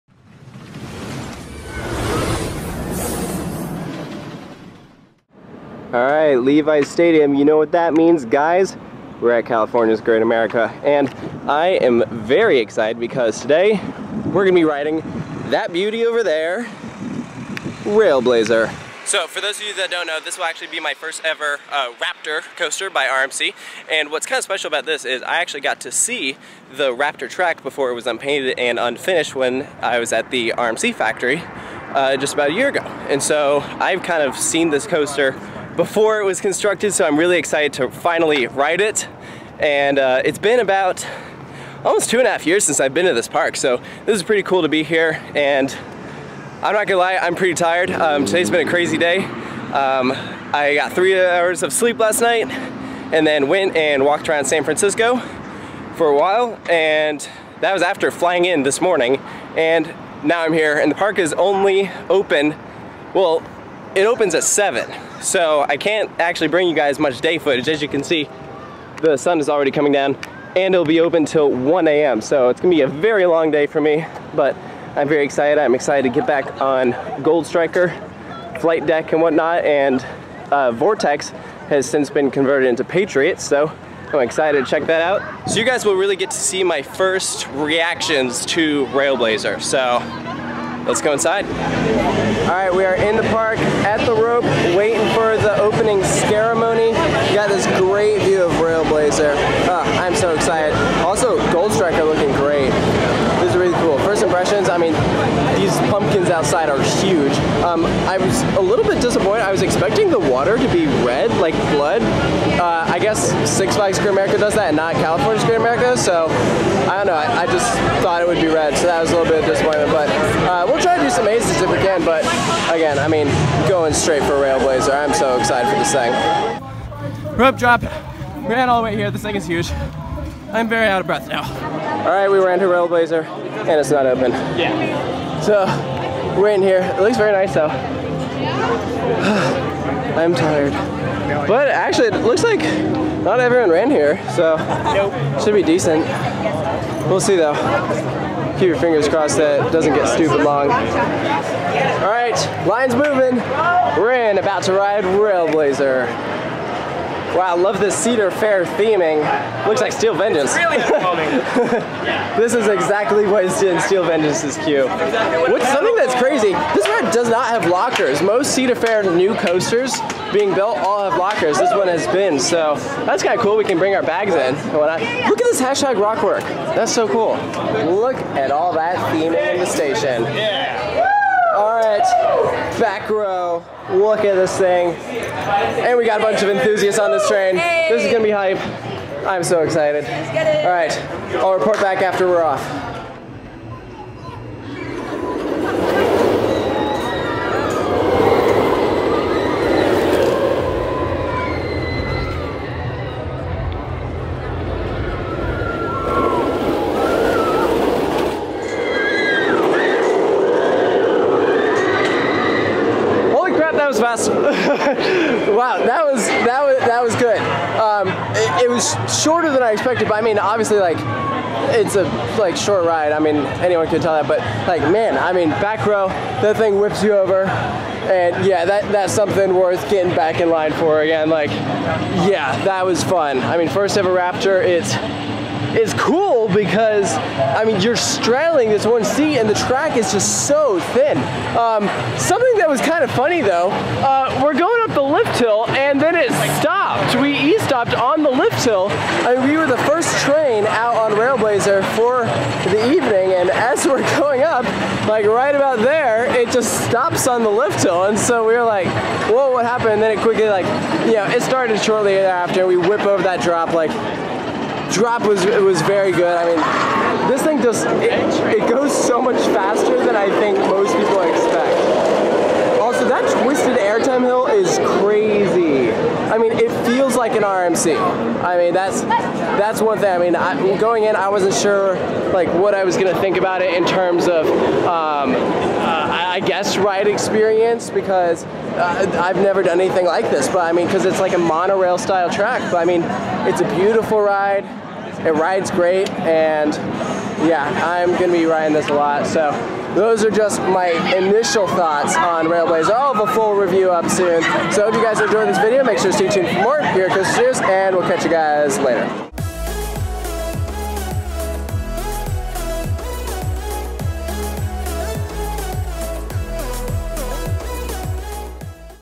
Riding Railblazer for the First Time California's Great America RMC Single Rail
Just over a year after I first walked on RMC track at their Idaho factory, I at last get to ride one of the Raptor tracks! New for 2018 is Railblazer, a single rail roller coaster at California's Great America. In this video, you'll get to hear my initial reaction to riding it immediately after I get off!

MY GEAR:
AT2020
Miracle Sound Lavalier